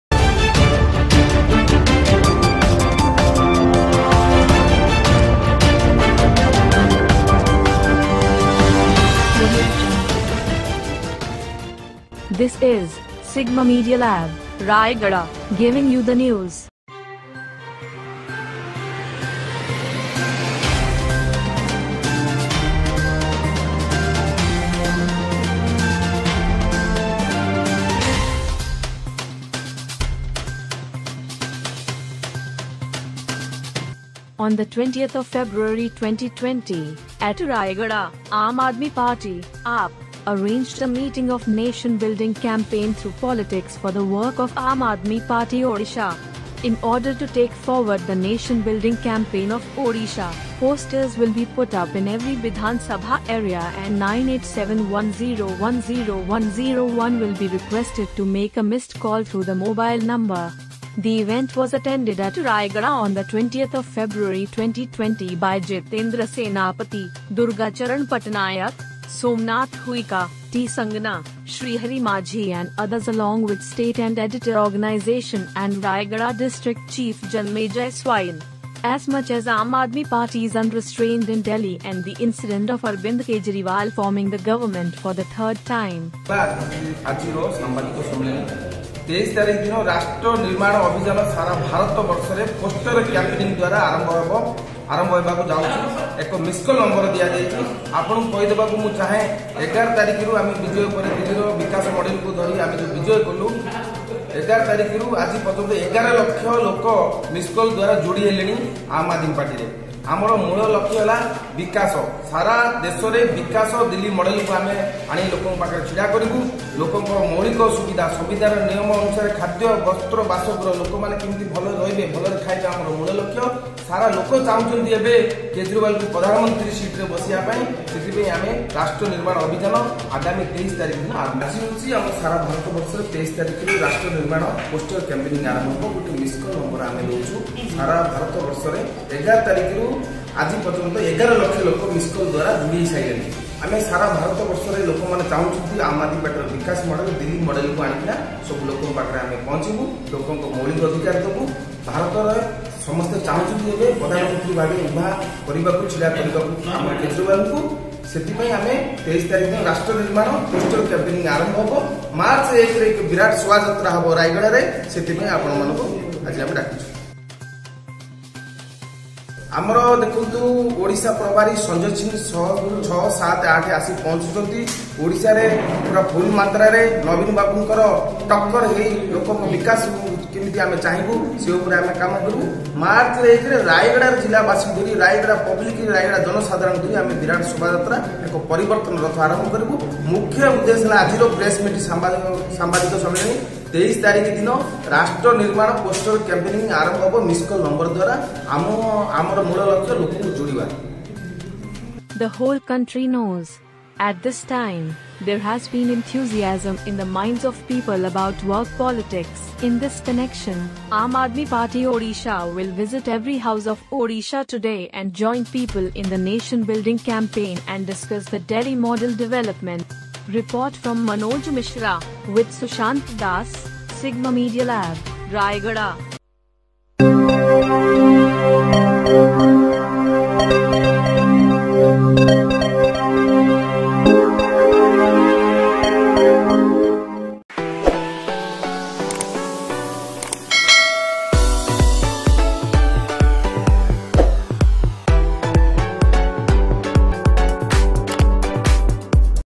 20.02.2020. A Press Meet for Nation building campaign through politics for the work of AAP, Odisha
In Odisha, this campaign fabric will start from the 23rd of March and will continue till the 23rd of March. With this goal, the party has aimed to deliver the Aurobind Kejriwalka Bikash Model to every house of Odisha. With this, ordinary people are being called to add work to politics. Through this, politics can be done for the problems of ordinary people in the whole country like Delhi. Through this campaign, people will be compared to Delhi and Odisha. One thing will be proved in this, how the politics of work can bring changes in the general public.

The Aam Aadmi Party started the nation-building campaign in the whole country after the victory of the Aurobind Kejriwal government in Delhi on 11th. The politics of the work of the watch was such a support in the whole country, within 24 hours, 11 lakh people participated in the nation building campaign by making a missed call in this number. The politics of Delhi's work was such a support in the whole country, within 24 hours, 11 lakh people participated in the nation building campaign by making a missed call in this number. The reason for this is that the change of various problems of schools, hospitals, water, electricity, women safety and common people has attracted people from the government side in Delhi. Today, many states in the whole country have adopted the model of development of Delhi. Some states have been seeking education in Delhi and some have taken Mohalla Clinic of Delhi in the health sector. Its effect is falling on people all over the country. They have turned towards politics of caste and religion and have waited for politics of work. Everyone wants to know the best model of Delhi. This is the reason for many people joining Aam Aadmi Party's nation building campaign.

In order to take forward the nation-building campaign of Odisha, posters will be put up in every Bidhan Sabha area and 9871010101 will be requested to make a missed call through the mobile number. The event was attended at Rayagada on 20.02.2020 by Jitendra Senapati, Durga Charan Patattanayak, Somnath Huika, T. Sangana, Srihari Majhi and others along with State & Editor (Organization) & Rayagada District Chief Mr. Janmajay Swain.

REPORT:
Editor: Manoj Mishra & Raygada Corespondent: Susant Das
SIGMA MEDIA LAB, RAYAGADA